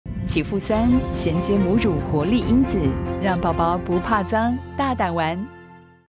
【啟賦3 HMO+ 讓寶寶不怕髒大膽玩】 6s
啟賦3 HMO+讓寶寶放心大膽玩髒髒！
啟賦3 研究理念源自母乳活性，
全球首創HMO+銜接母乳五大活力因子，
開啟寶寶活力關鍵，不怕髒大膽玩！

【銜接母乳5大活力因子】
✅ HMO：幫助調整體質
✅ MOS：研究發現*MOS是保護寶寶的強力後盾
✅ sn-2：促進關鍵營養吸收
✅ α 蛋白：幫助健康成長
✅ 核苷酸：維持消化道機能
*Elvira E. et al. Am J Clin Nutr 2021;00:1-12
*A. Zivkovic, D. Barile. Adv. Nutr. 2: 284–289, 2011